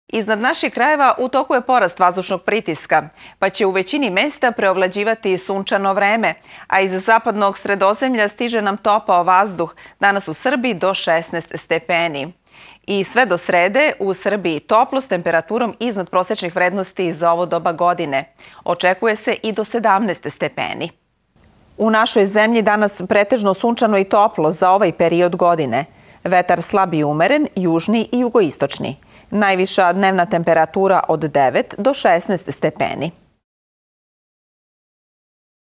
Vremenska prognoza do kraja dana za 6. januar 2018.
Pretežno sunčano i toplo za ovaj period godine.

Vetar slab i umeren južni i jugoistočni. Najviša dnevna temperatura od 9 do 16 stepeni.